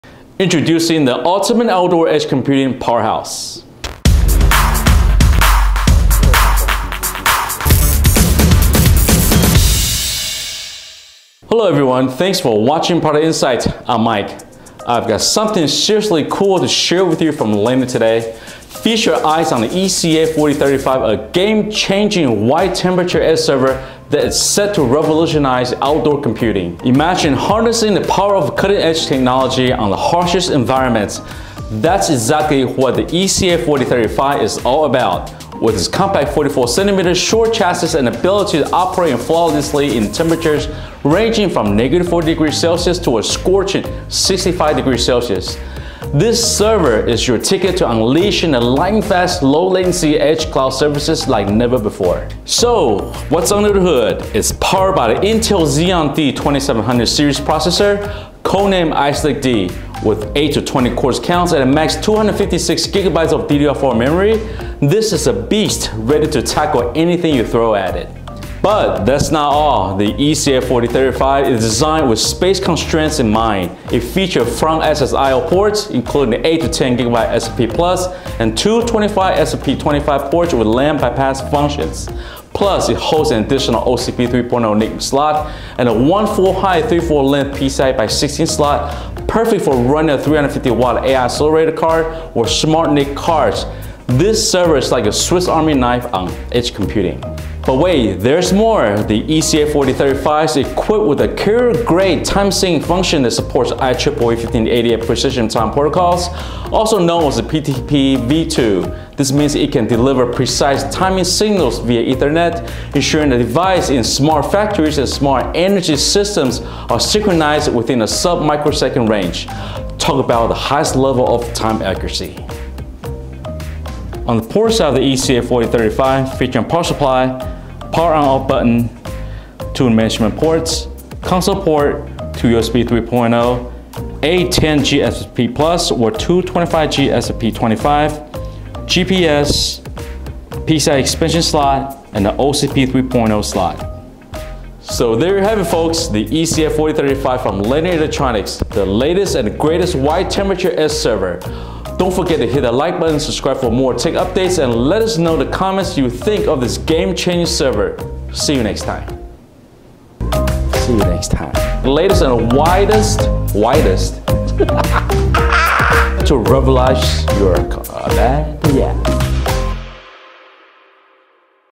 Product Insight EP57: The Ultimate Outdoor Edge Computing Powerhouse
Lanner’s ECA-4035 is a game-changing wide temperature edge server, the Intel Xeon D2700 Series processors, codenamed Ice Lake D. With 8 to 20 core counts and a max 256GB DDR4 memory. Moreover, it is equipped with a carrier-grade time-sync function that supports IEEE-1588 precision time protocols.

The ECA-4035 features front-access IO ports, including 8x 10G SFP+ and 2x 25G SFP28 ports with LAN bypass function. Plus, it hosts an additional OCP 3.0 NIC Slot and a 1x FH 3/4L PCIe*16 slot, perfect for running a 350W AI accelerator card or Smart NIC cards.

ECA-4035
High Performance Edge Computing Appliance With Intel Xeon® D-2700 Multi-core Processor